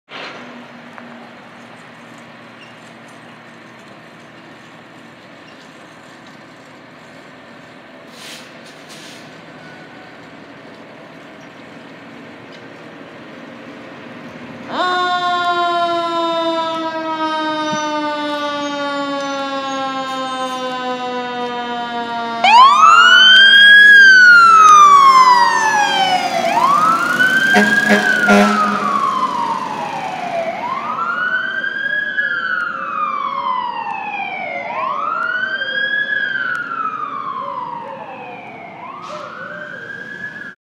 Heartland Engine 11 responding
HRT E11 responding to a level 1 medical in their new engine

Filmed 1/2/2022